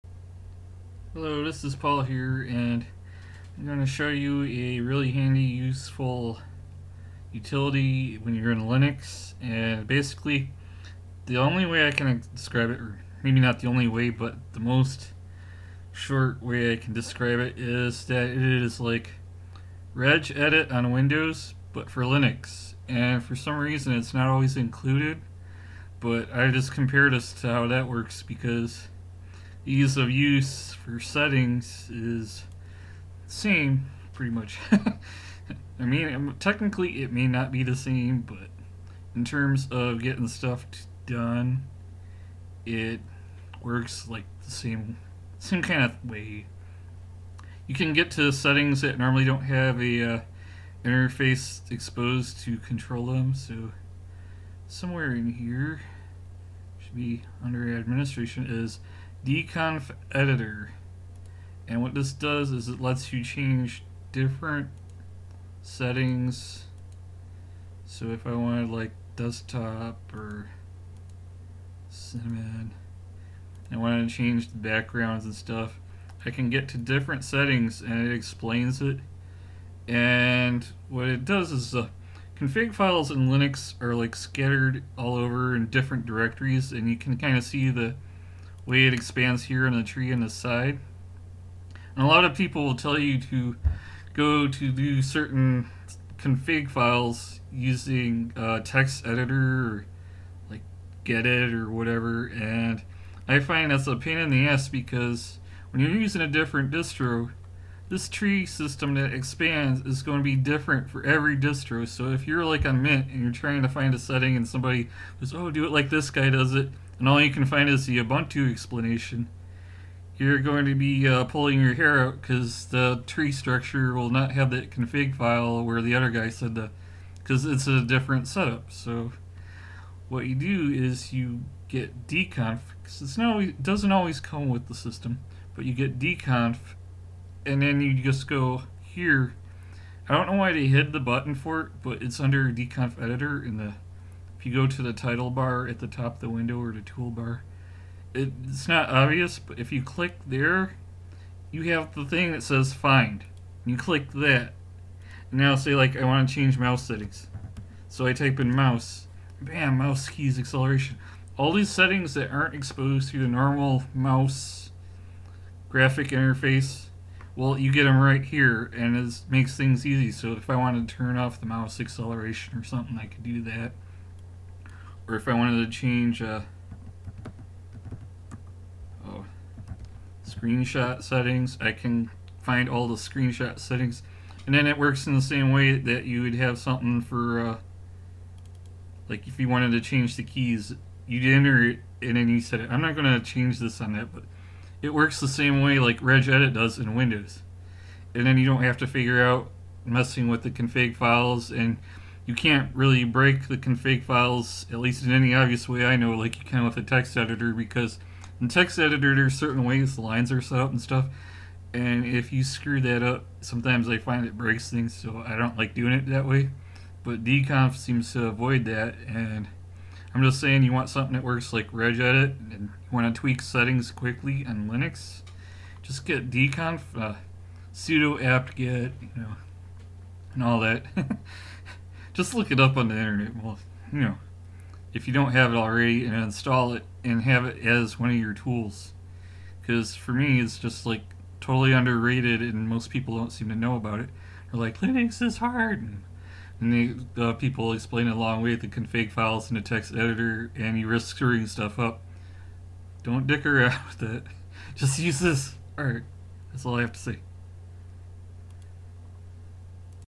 Regedit for Linux
I'm talking about the completely underrated administrative utility known simply as "Dconf". (Or d-config, depending who you ask.) Yeah I know it's technically not the same as Regedit on Windows (because reasons), but for most practical purposes it's used the same way. You need to quickly tweak some system setting that doesn't have a readily exposed GUI interface, and this seems to be the most handy and easy way to do it. The find feature is the most handy, because the majority of the time it doesn't need you to know the particulars of your specific distro or desktop environment.